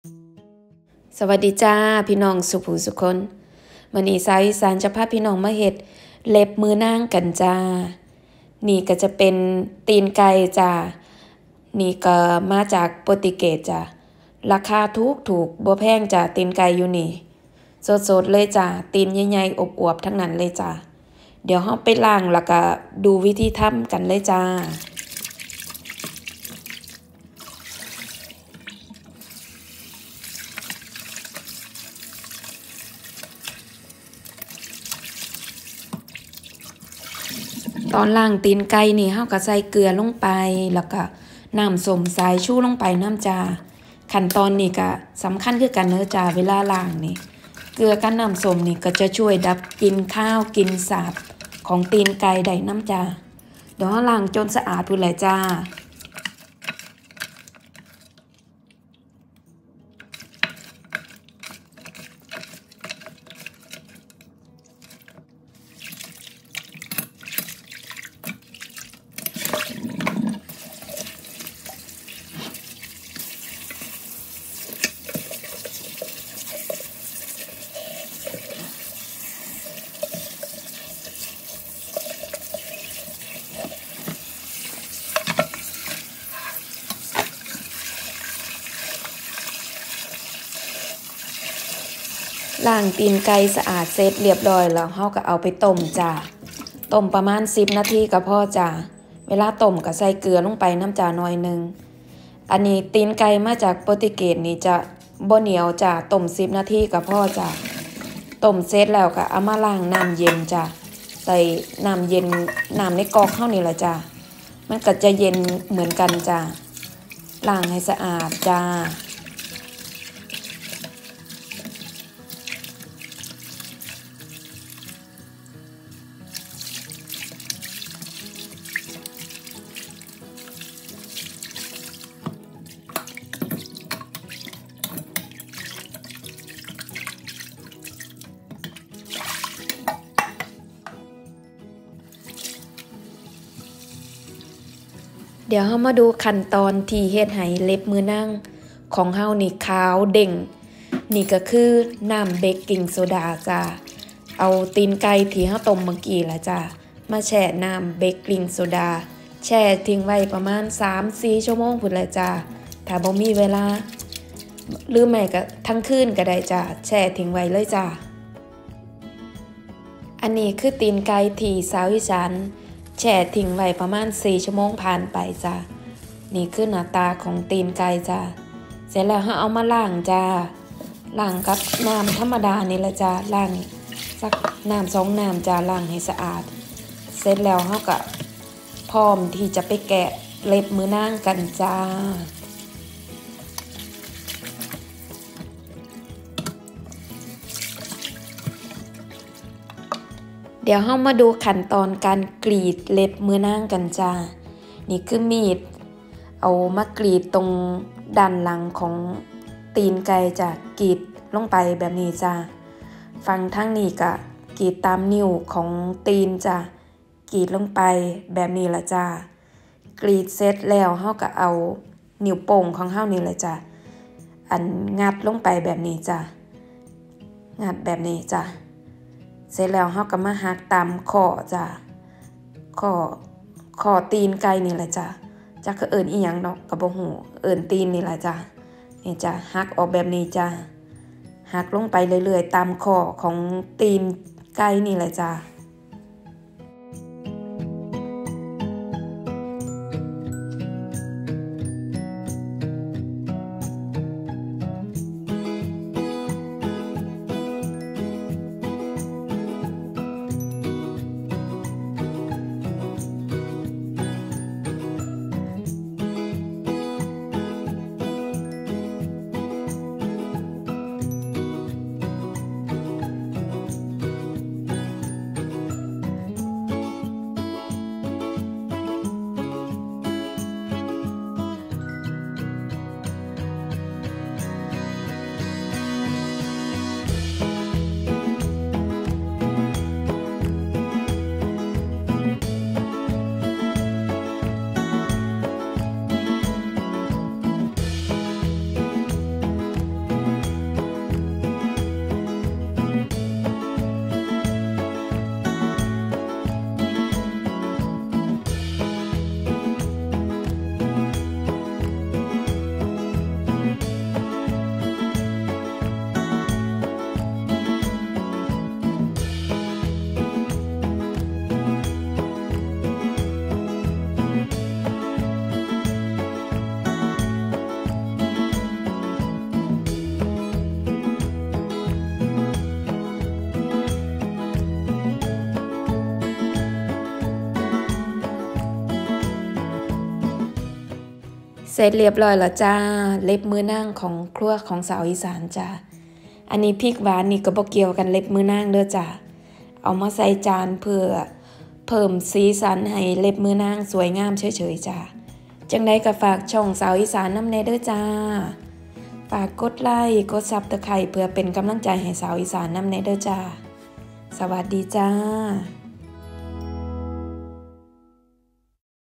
วิธีแกะเล็บมือนาง ขาว อวบ เด้งๆ
วิธีทำเล็บมือนาง
1.ตีนไก่
2.เกลือ
3.เบรคกลิ้งโซดา

คั้นตอนทำ นำตีนไก่ ไปล้างให้สะอาด
แล้วนำมาต้มประมาณ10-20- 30 นาที แล้วแต่ตีนแต่ล่ะพื้นที่
เสร็จแล้วนำมาล้างด้วยน้ำเย็น
แล้วนำตีนไก่ไปแช่น้ำเบรคกลิ่งโซดาประมาณ 3-4ชั่วโมง เสร็จนำมาแกะ ตีน ให้ทำตามในคลิป วิดีโอใด้เลยจ้า คั้นตอนทำ